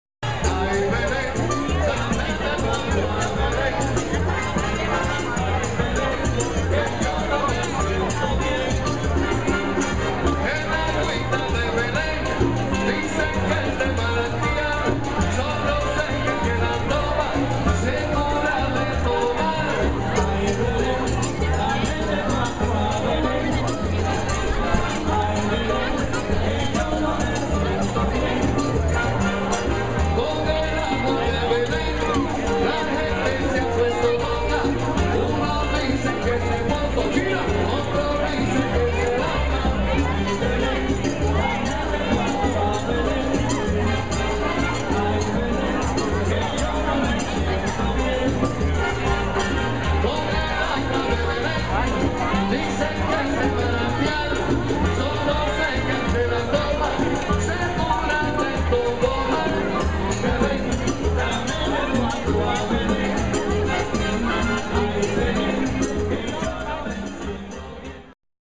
AFRO LATIN JAZZ
Actuacion del pianista ganador del Grammy, Arturo O'Farril y su orquesta.